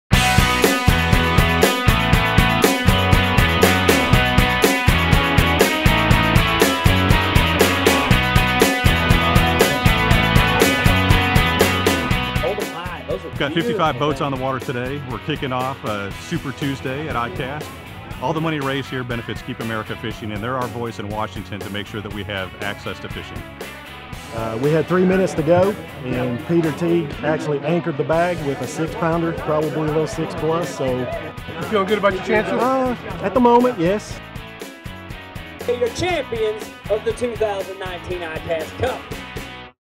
ICAST 2019 Recap - ICAST Cup
A little heat didn't stop the bass from biting during the 2019 ICAST Cup. Congratulations to the Lew's Fishing Team of John Carman, Peter Thliveros and Matthew Mattingly with the first place weight of 20 pounds, 4 ounces. With the win, Team Lew's is our first back-to-back champions!

The event is presented by FLW.